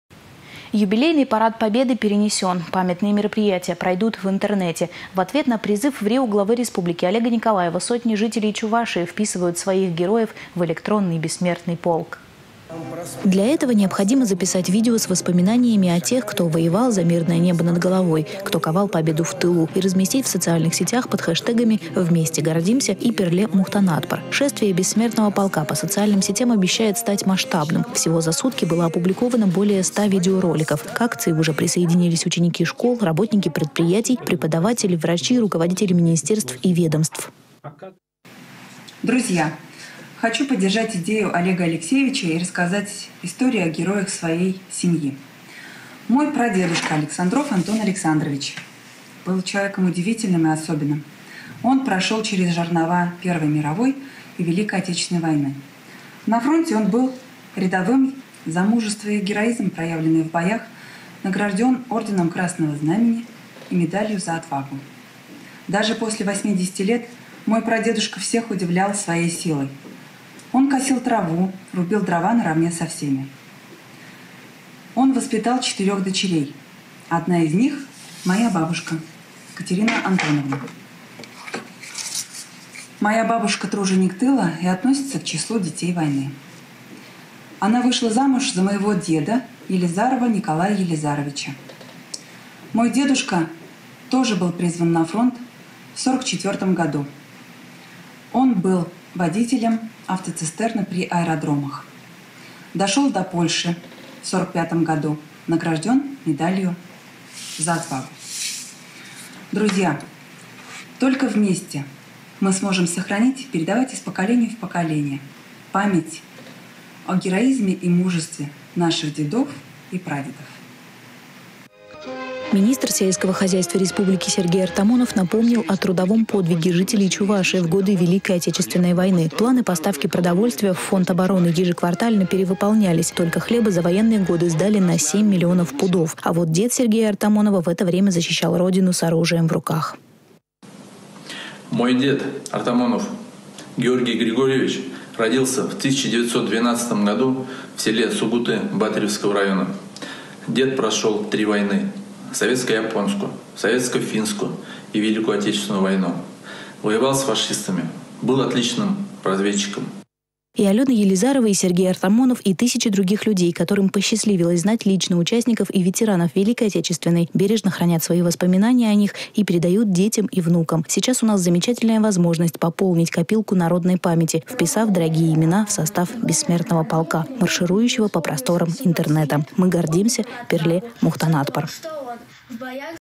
В Чувашии проходит акция
Юбилейный парад Победы перенесён, памятные мероприятия пройдут в интеренете. В ответ на призыв врио Главы республики Олега Николаева сотни жителей Чувашии вписывают своих героев в электронный Бессмертный полк.